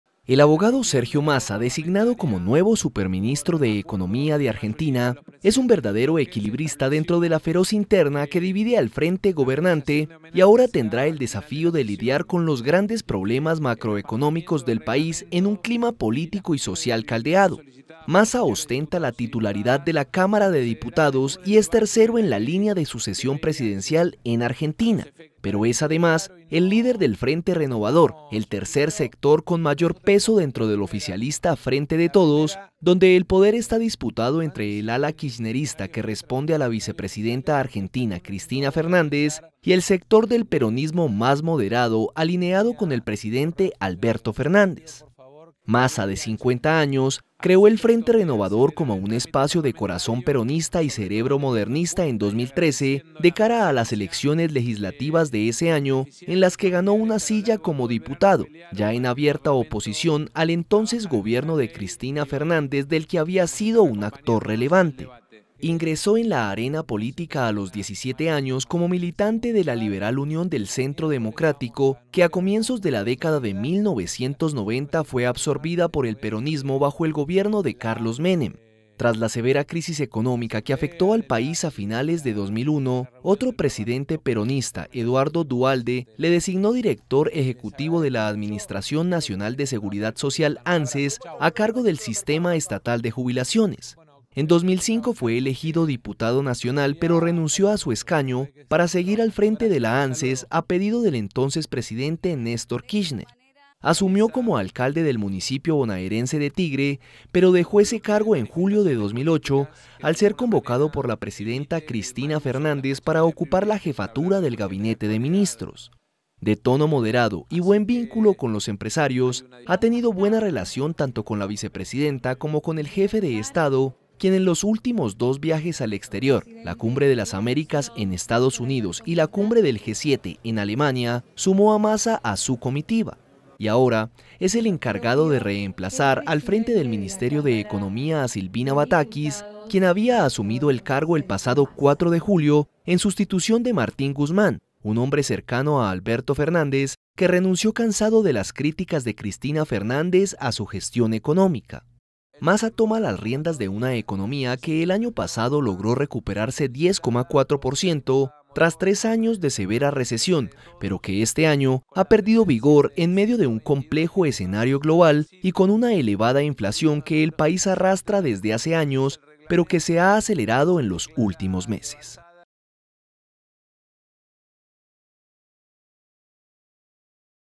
Fernández nombra a un nuevo ministro de Economía en Argentina, el tercero en un mes
Sergio Massa, de 50 años, llegará en sustitución de Silvina Batakis, que había asumido el 4 de julio tras la renuncia de Martín Guzmán en medio de fuertes tensiones políticas en el seno del oficialismo

Vídeo: EFE